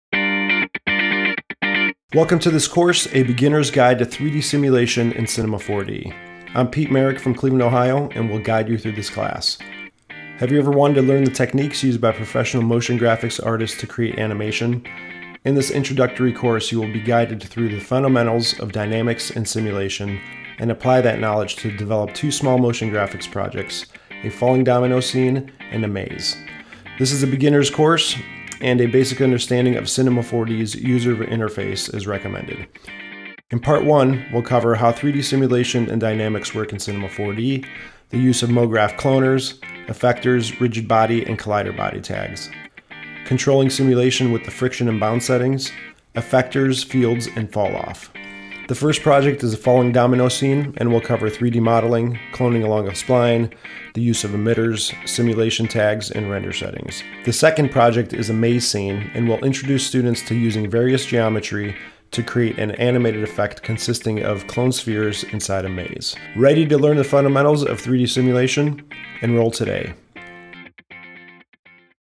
A Beginner’s Guide to 3D Simulation in Cinema 4D
An introductory project-based course for the aspiring 3D motion graphic artist.

In this series of lessons we will cover the fundamentals of 3D Simulation to help you gain a basic understanding of the tools and techniques used in the industry.

Part 01: Introduction to 3D Simulation Techniques

 • Overview of the Mograph cloners, random effector, rigid body tag, collider body tag, friction and bounce settings
 • Experimentation with parametric primitives and various geometry for different dynamic effects
 • Splines, lathe nurbs, rigid body tag, collider body tag, friction and bounce settings
 • Create a vase being filled with spheres
 • Using an emitter with a rigid body sphere to start a simulation
 • Work with the initial linear velocity
 • Overview of the random, formula, push apart and step effectors
 • Use of spherical, cylindrical, cone, linear, radial fields and falloff settings

Part 02: Domino’s Project

 • Model the domino’s with splines and extrude nurbs
 • Use domino images from the asset browser to texture the model
 • Use the selection field to apply materials to the front, back and side of dominos
 • Use of the MoGraph Cloner in object mode in conjunction with a spline to duplicate the dominos along a path
 • Adjust the cloner’s distribution settings for evenly spaced dominos
 • Use rigid body and collider body tags to set-up the simulation
 • Use and emitter to create the initial impact by setting the initial linear velocity for domino’s to fall
 • Refine bounce, friction and rigid body settings
 • Define final output render settings to complete the animation

Part 03: Maze Project

 • Model the 3D environment and add a collider body tag
 • Create a sphere inside a cloner and assign a rigid body tag to start the simulation
 • Model the maze geometry using parametric primitives
 • Create shelves, front and add collider body tags
 • Adjust the maze geometry to control the simulation
 • Duplicate the sphere cloner to add additional animation to the scene
 • Create sides to the maze to prevent spheres from falling outside of it’s borders
 • Use the display tag to hide geometry in the perspective view
 • Apply procedural Cinema 4D materials to the maze and spheres
 • Use gradients on the spheres and refine their parameters for added visual interest
 • Create a camera animation to follow the simulated sphere movement
 • Define render settings for final output